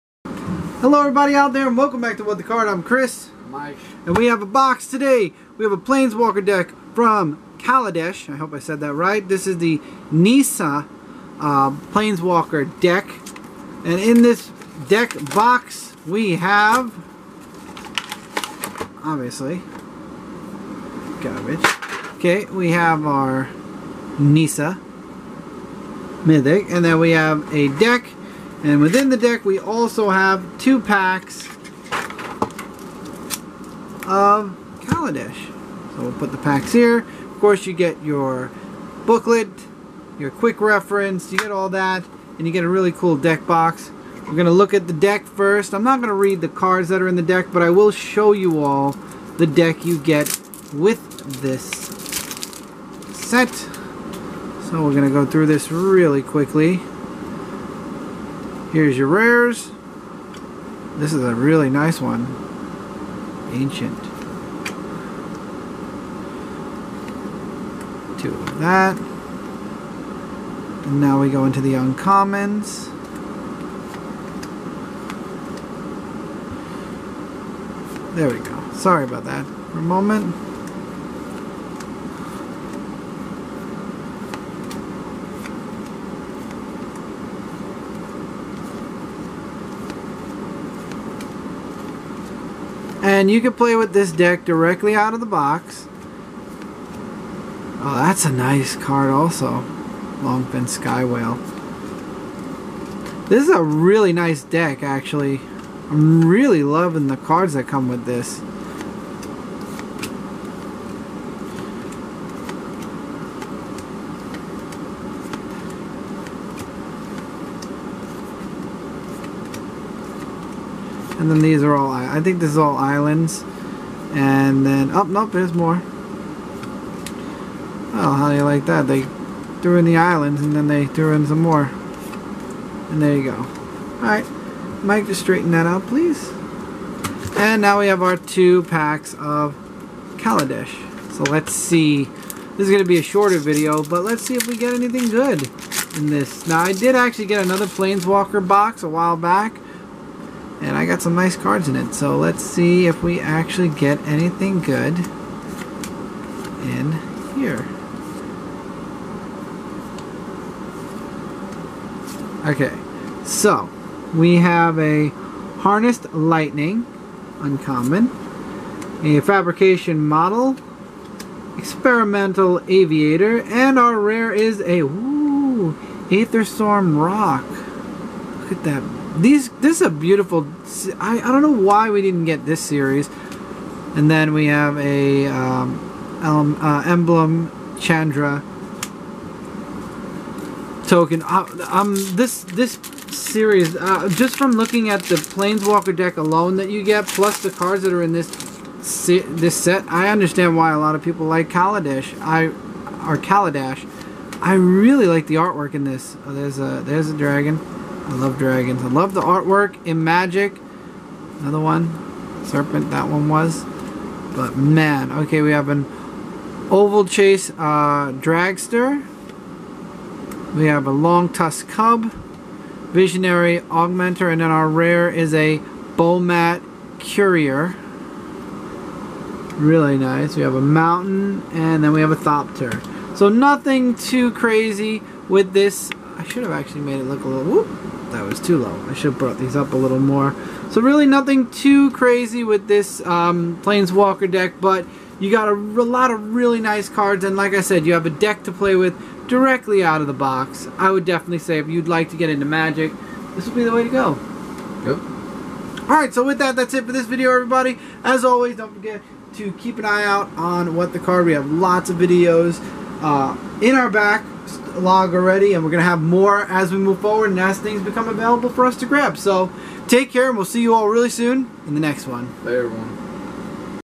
Magic: Nissa
Chris and Mike open the Magic The Gathering Nissa Kaladesh Planeswalker Deck